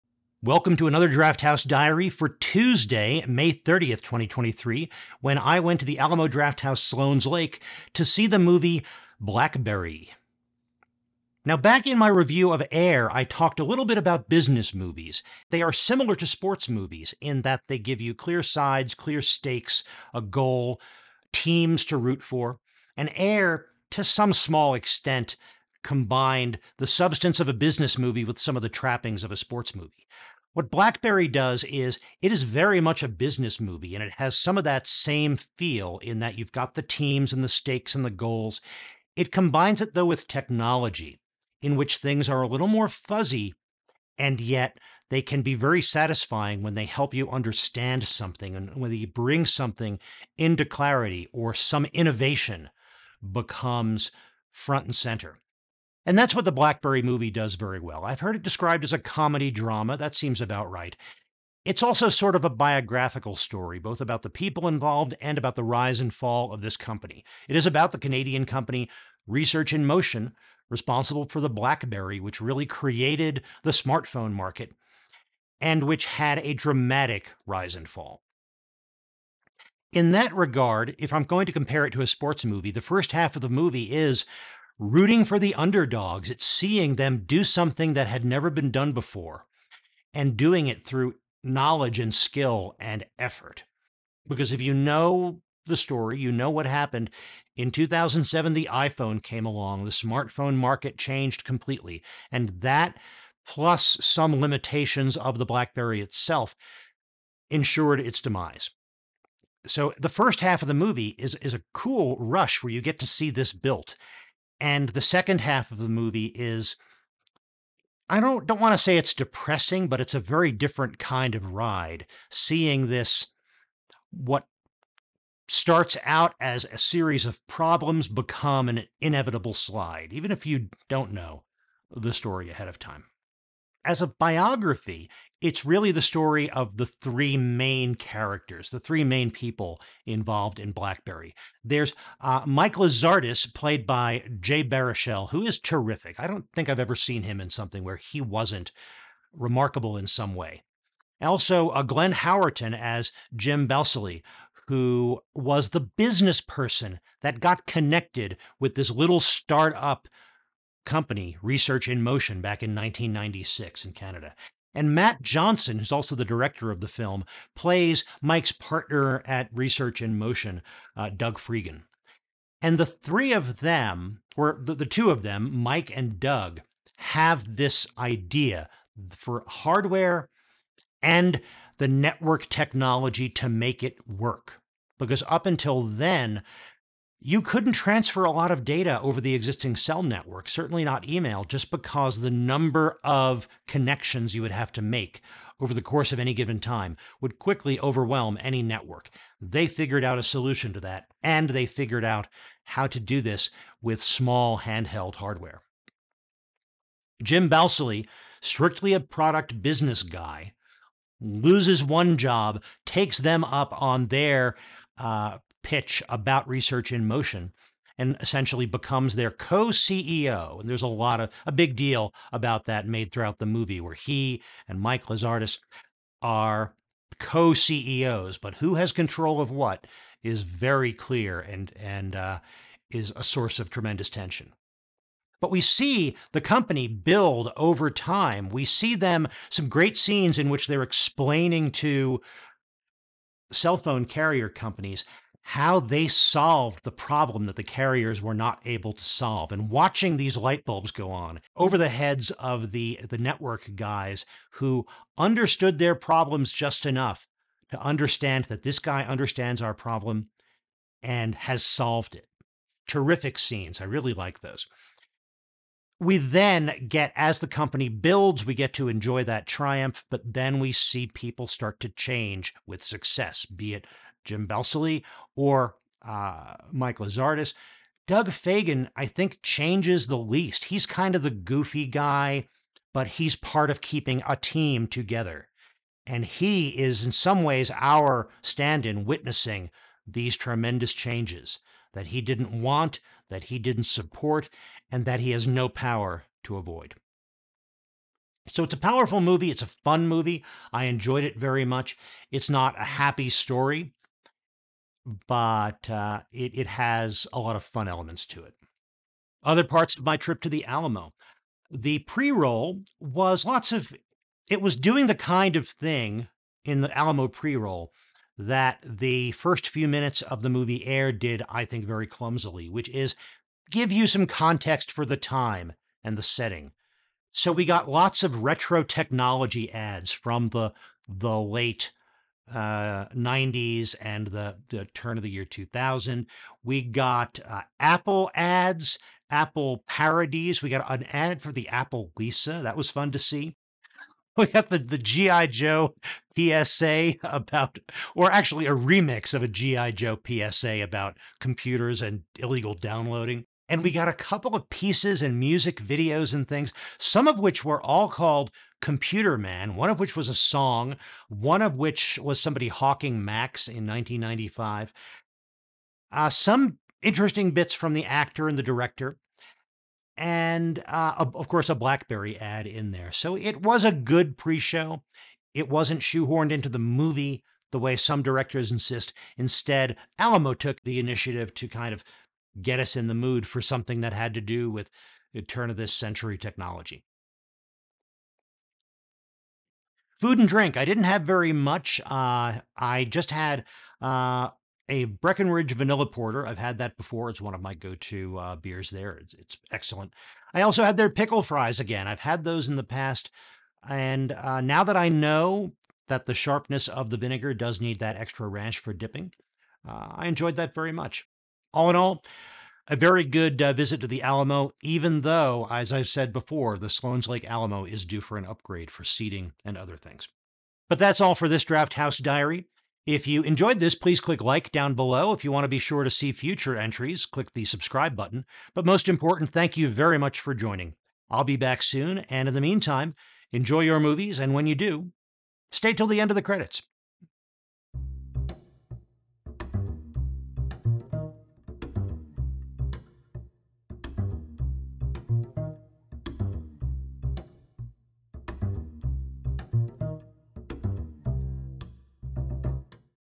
BLACKBERRY - Tech Company Biopic from the dawn of the smartphone age (Drafthouse Diary Movie Vlog)
Movie: BLACKBERRY
Food: Pickle Fries, and Breckenridge Vanilla Porter

In the Drafthouse Diary I review my visits to Alamo Drafthouse Cinemas. Reviews of the movies, and also the food, the famous Alamo pre-shows, and the whole moviegoing experience.